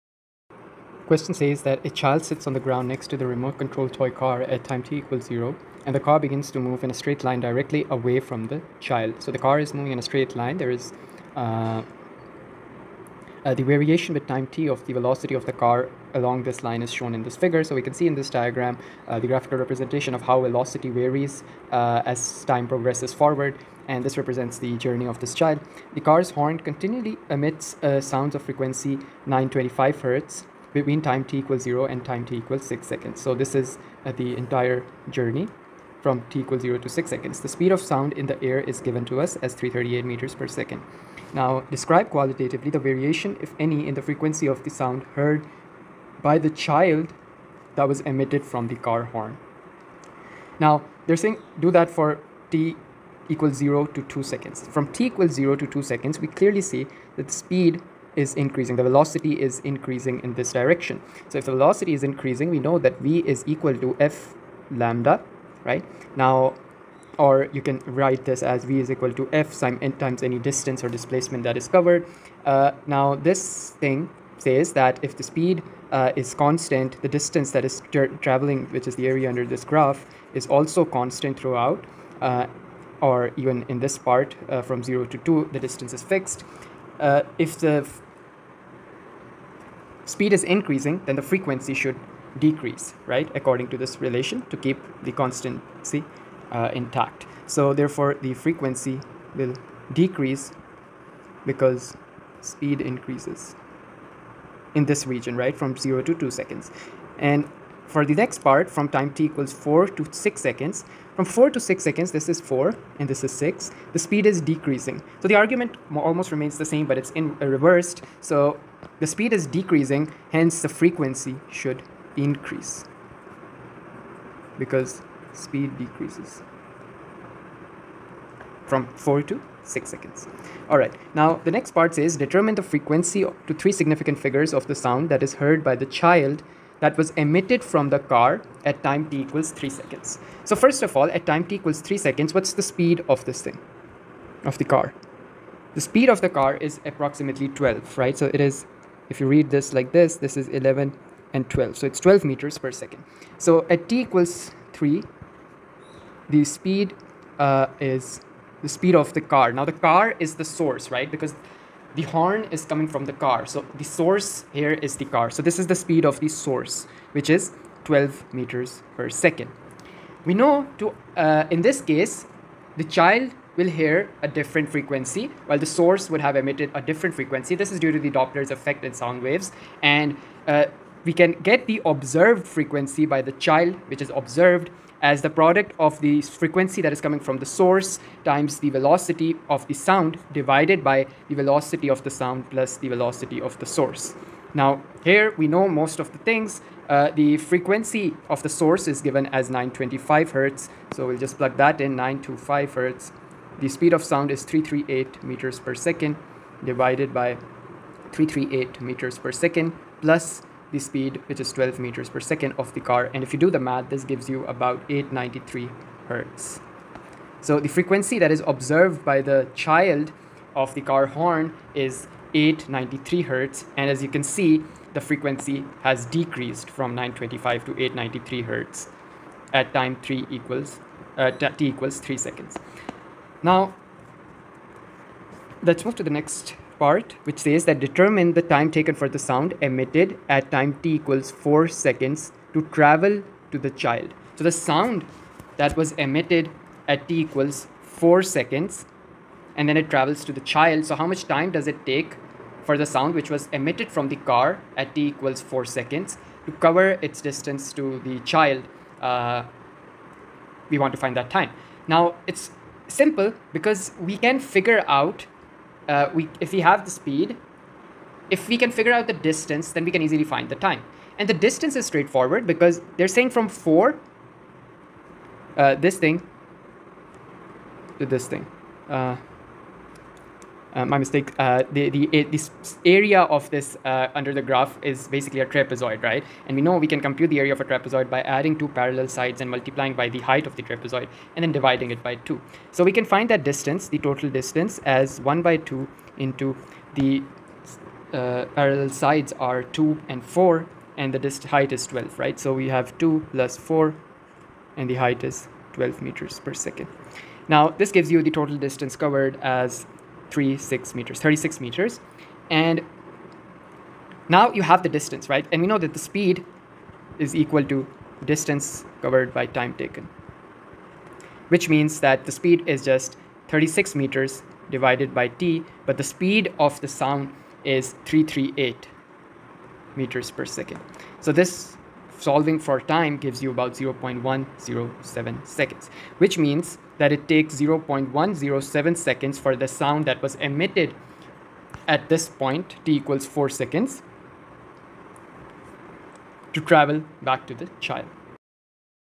AS Physics - 9702 P22 ON21 Q4 - Past Paper Question
A child sits on the ground next to a remote-controlled toy car. At time t = 0, the car begins to move in a straight line directly away from the child. The variation with time t of the velocity of the car along this line is shown in Fig. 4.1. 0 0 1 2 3 t/ s velocity /m s–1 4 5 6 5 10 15 Fig. 4.1 The car’s horn continually emits sound of frequency 925Hz between time t = 0 and time t = 6.0s. The speed of the sound in the air is 338ms–1. (a) Describe qualitatively the variation, if any, in the frequency of the sound heard, by the child, that was emitted from the car horn: (i) from time t = 0 to time t = 2.0s [1] (ii) from time t = 4.0s to time t = 6.0s. [1] (b) Determine the frequency, to three significant figures, of the sound heard, by the child, that was emitted from the car horn at time t = 3.0s. frequency = Hz [2] 11 © UCLES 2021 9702/22/O/N/21 [Turn over (c) Determine the time taken for the sound emitted at time t = 4.0s to travel to the child. time taken = s [2]
Book a FREE demo class for Oct Nov 2022 session through the